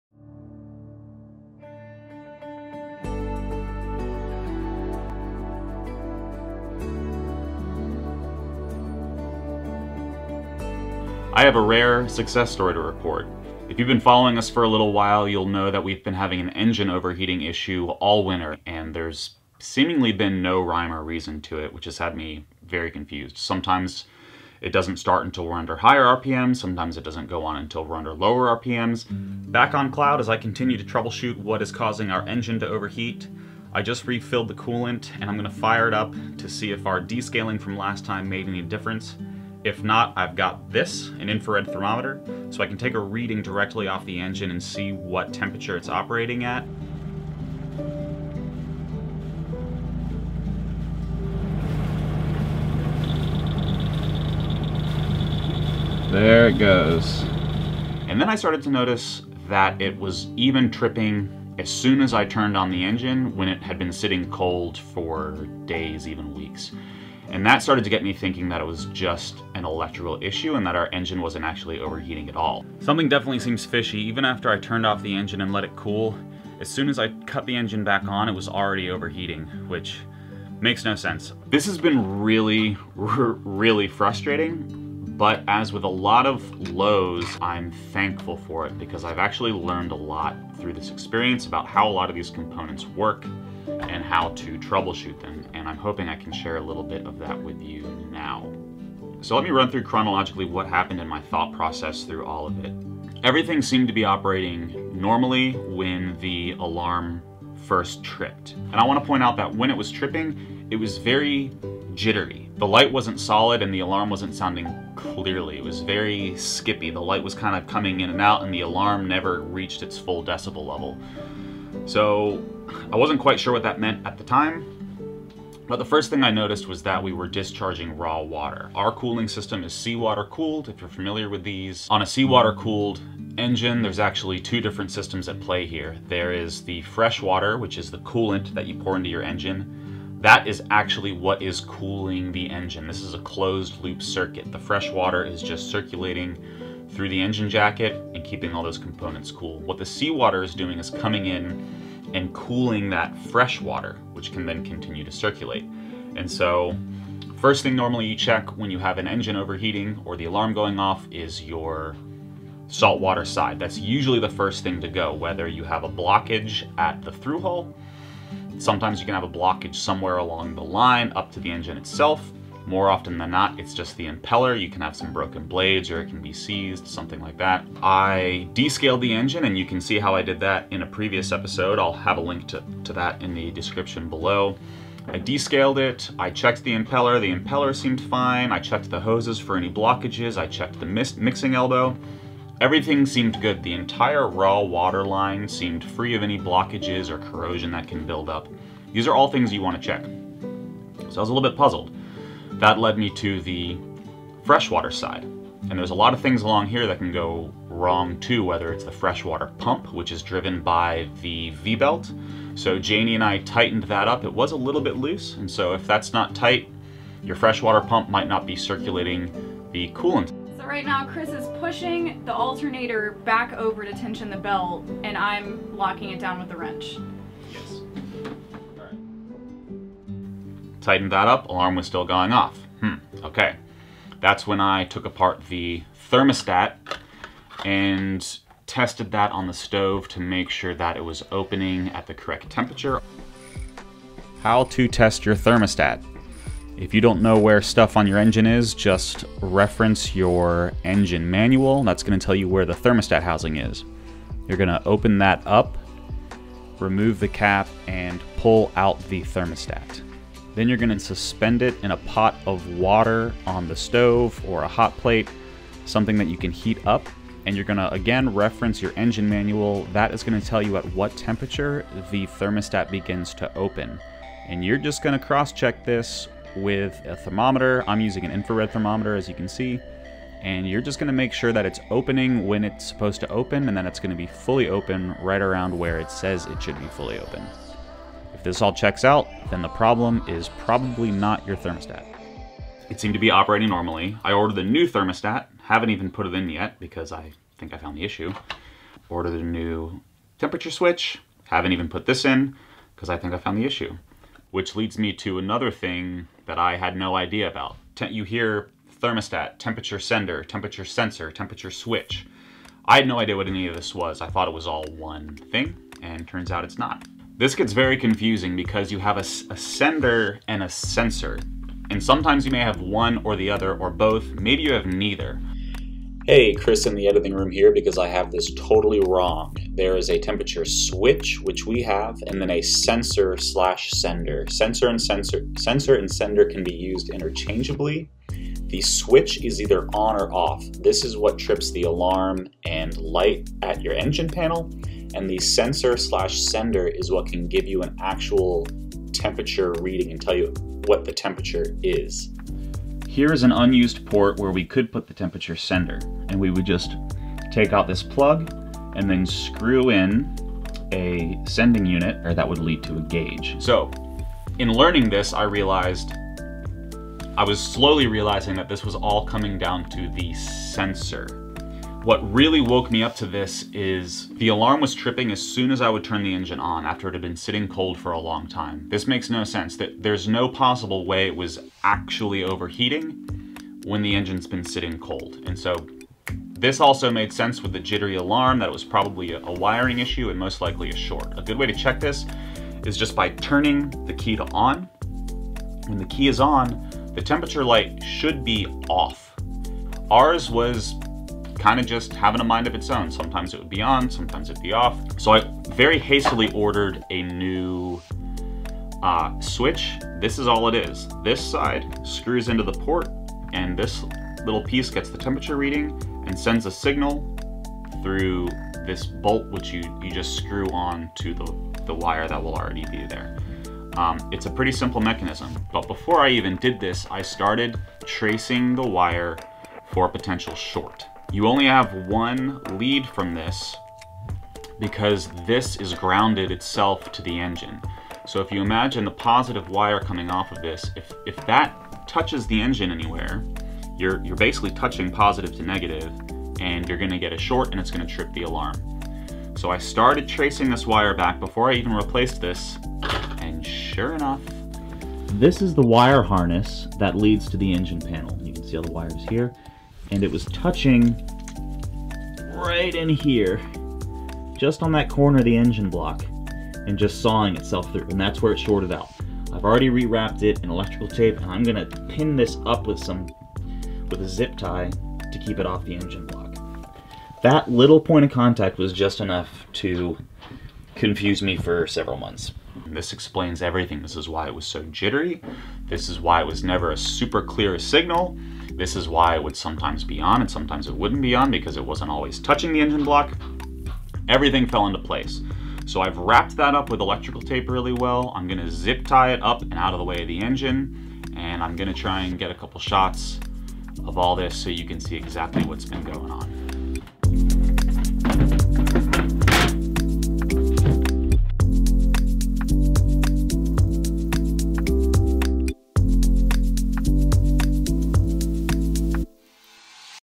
The Ultimate Guide to an Overheating (Or Not) Engine | 05
Success!!! Chris has figured out why our temperature alarm has been sounding and walks through his entire troubleshooting process. If you've ever had your sailboat engine overheat, this should be a great guide on where to look!

⪼Video References
Infrared Thermometer

MB01OOCTERZWNVJ
MB019S1JOTSO8BL